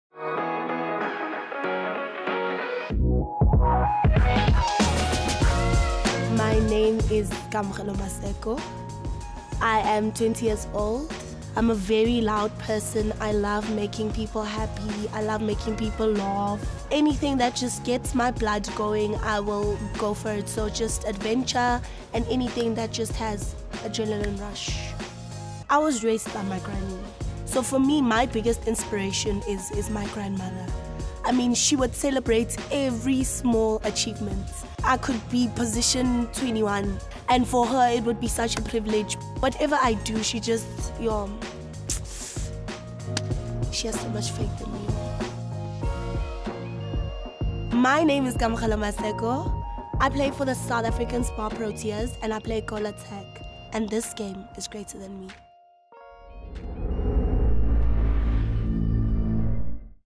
Supersport Netball Interview n3
'Netball to the world'

Project Name:
Supersport Netball TVC Spots

Client:
DSTV

Challenge:
DTSV needed multiple TVC spots created in anticipation for the 2022 Netball world cup.

Collaborators:
Ogilvy South Africa